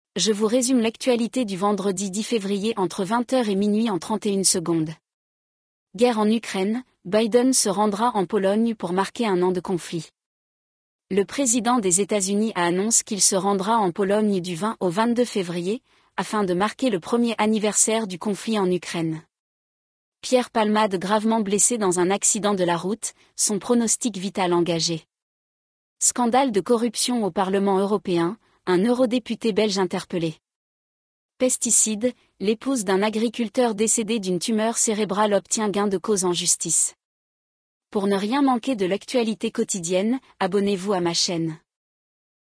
Résumé de l'actualité du jour : Vendredi 10 février entre 20h et minuit
- Guerre en Ukraine : Biden se rendra en Pologne pour marquer un an de conflit
- Pierre Palmade gravement blessé dans un accident de la route, son pronostic vital engagé
- Scandale de corruption au Parlement européen : un eurodéputé belge interpellé
- Pesticides : l’épouse d’un agriculteur décédé d’une tumeur cérébrale obtient gain de cause en justice